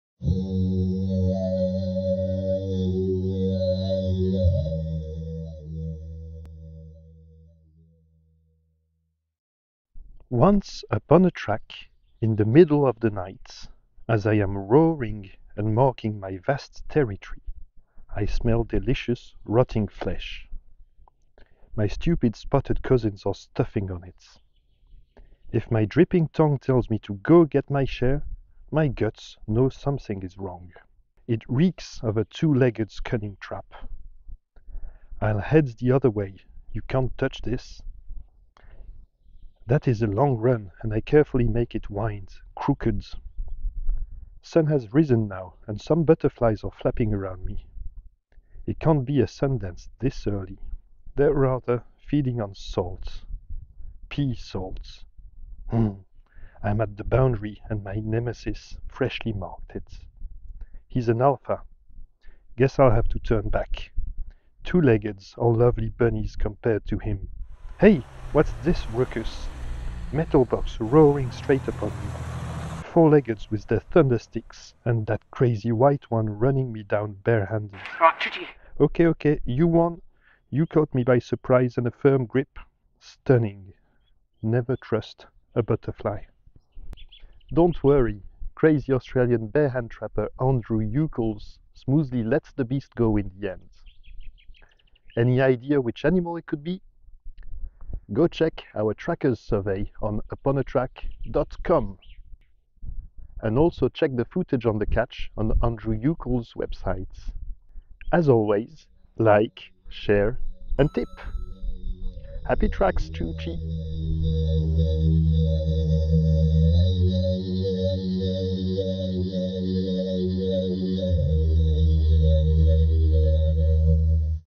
011 - Butterfly Effect
BUTTERFLY EFFECT is "Upon a Track"s 11th Story.
Tracking has been made by Andrew Ucles, in March, Africa.

Try to guess which animal is the narrator of this Wild Story before it unravels, then check our Tracking survey

Story, concept, video editing, music, drawing by Francis Collie.
An original creation by Je Suis La Piste association.